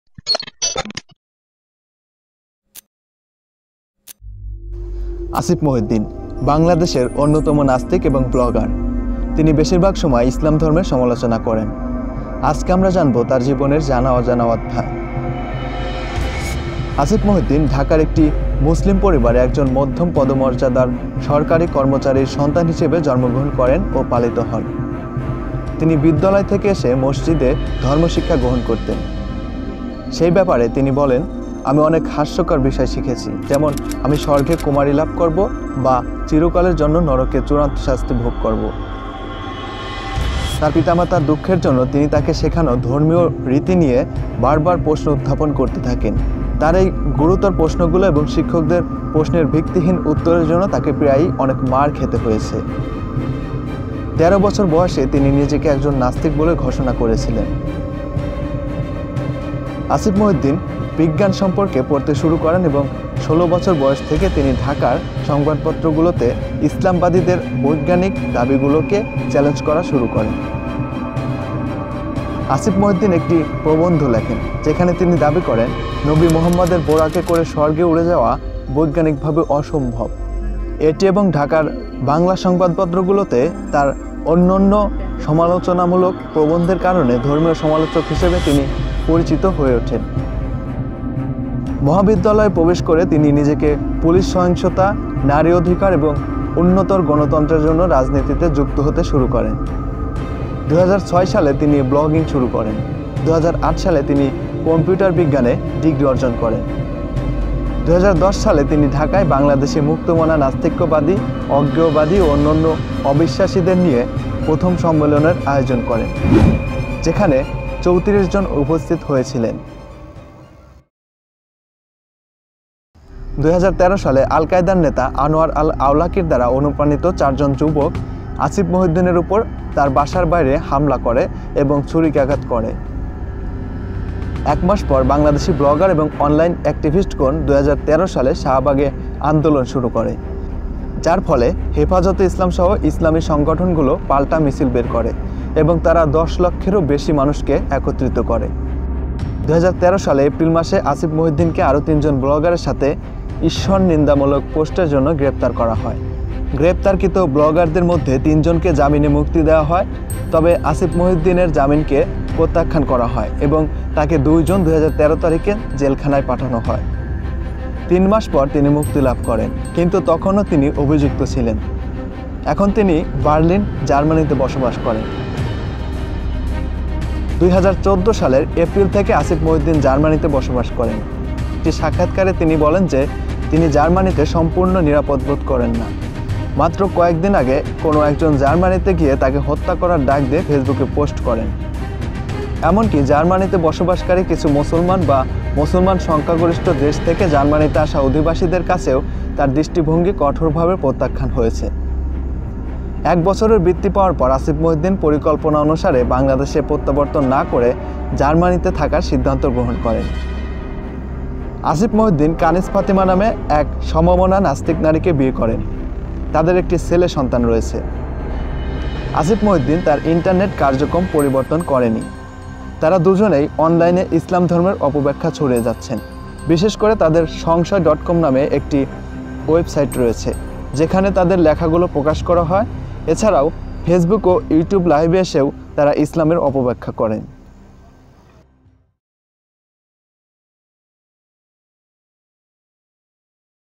১৩ বছর বয়সে নিজেকে নাস্তিক ঘোষণা: আসিফ মহিউদ্দিন | Atheist & Blogger | Asif Mohiuddin | BioPic
বাংলাদেশী নাস্তিক @asif.mohiuddin আসিফ মহিউদ্দিন এর জীবনী | Biography Of Asif Mohiuddin | BioPic Channel

Information References: Various books, newspapers, Wikipedia, websites and others.

Other Channels.

আমাদের ভিডিওগুলো সুন্দর ভাবে Playlists🎬 আকারে দেওয়া আছে..